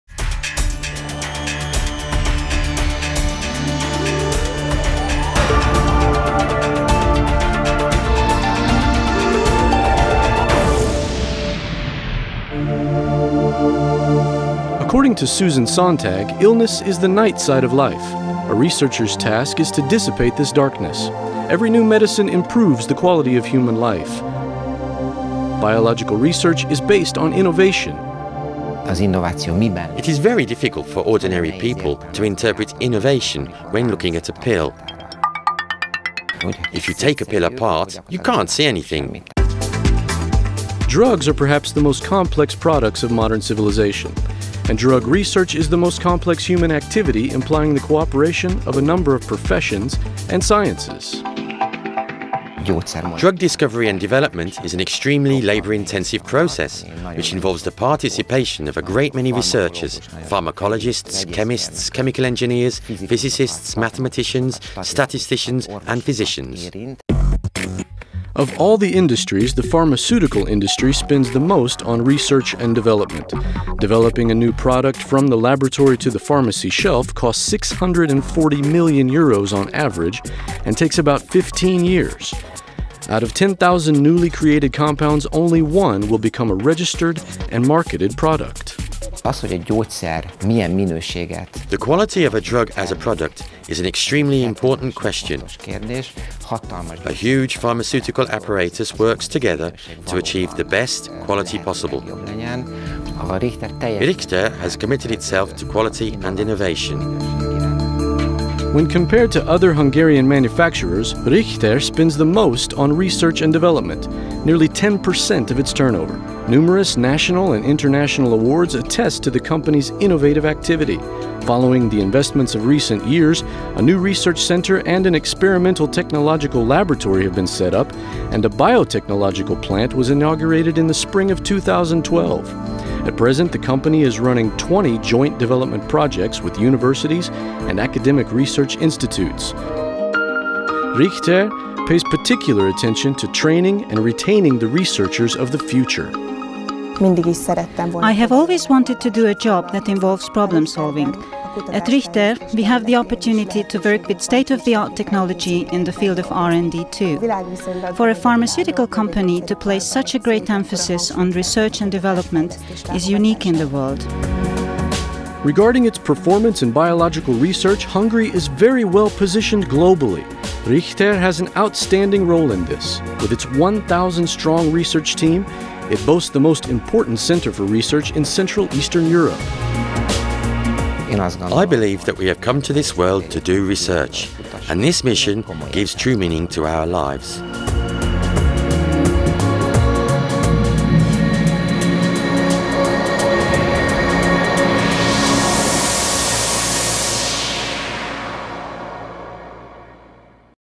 Gedeon Richter - Investigação & Desenvolvimento
Vários prémios nacionais e internacionais atestam a atividade inovadora da empresa. Na sequência dos investimentos realizados nos últimos anos, foi criado um novo centro de investigação e um laboratório tecnológico experimental e, na primavera de 2012, foi inaugurada uma unidade biotecnológica.

Atualmente, a empresa tem em curso vinte parcerias em projetos de desenvolvimento com universidades e instituições académicas ligadas à investigação.